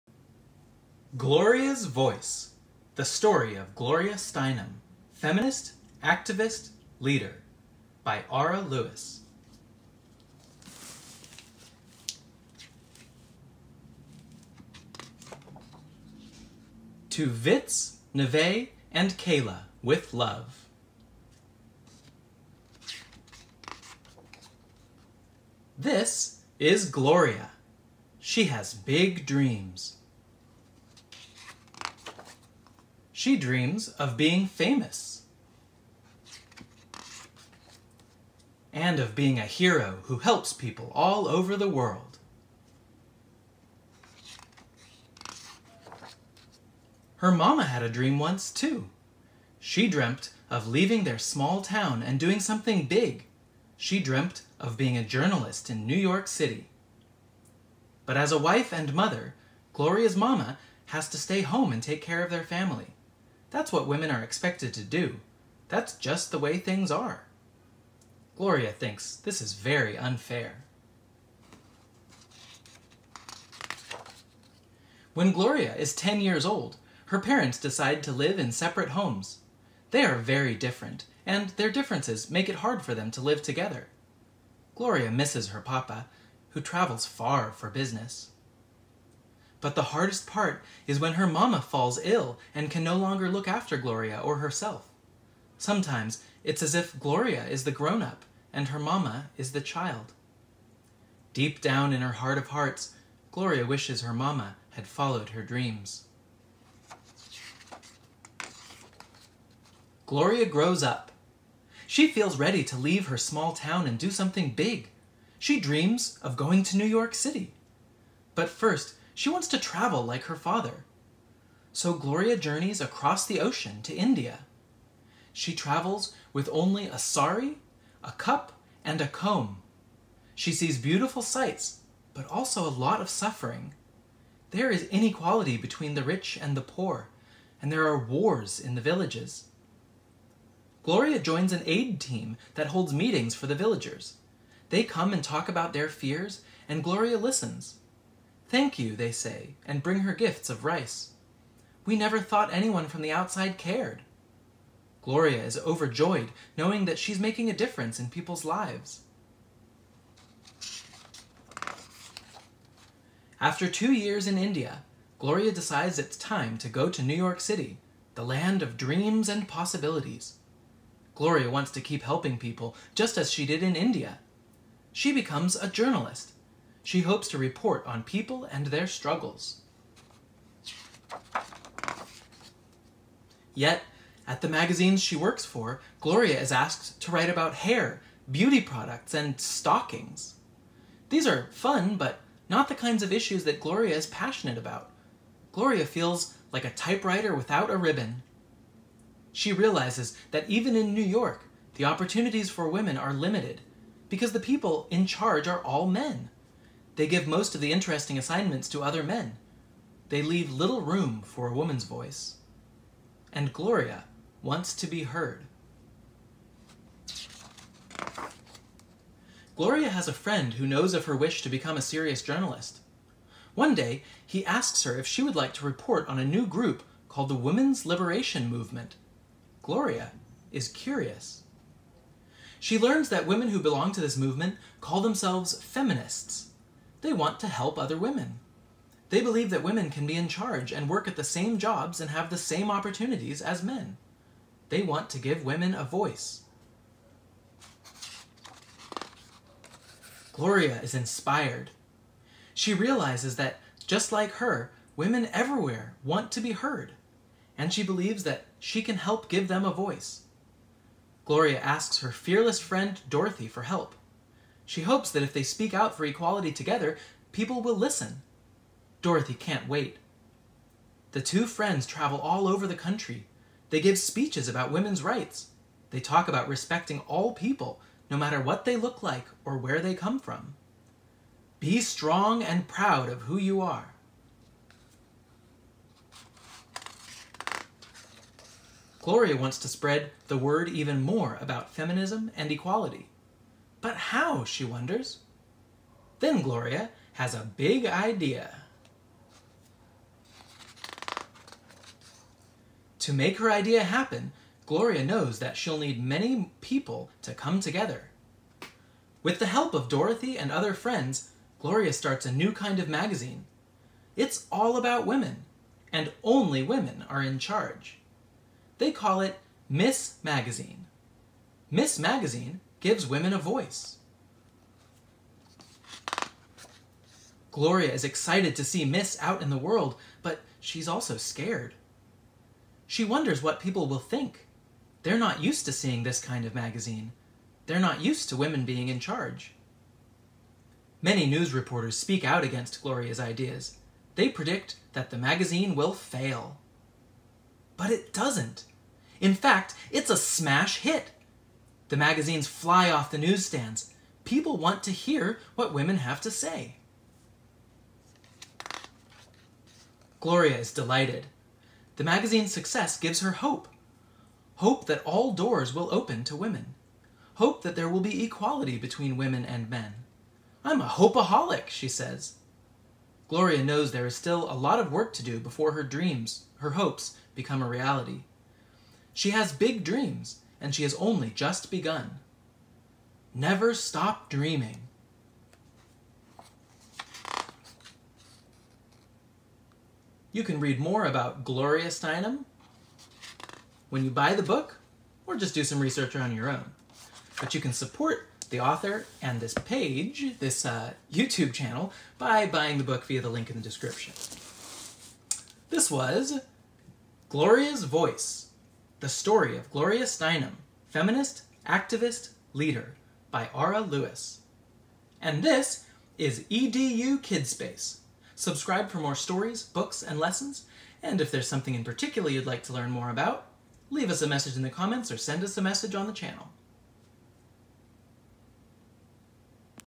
Gloria’s Voice: the Story of Gloria Steinem—Feminist, Activist, Leader; by Aura Lewis
How a hero for rights and equality helped many women find their voice.

*I am a Montessori Elementary teacher and educational content-developer, who started a youtube channel while on pandemic lockdown in New York City.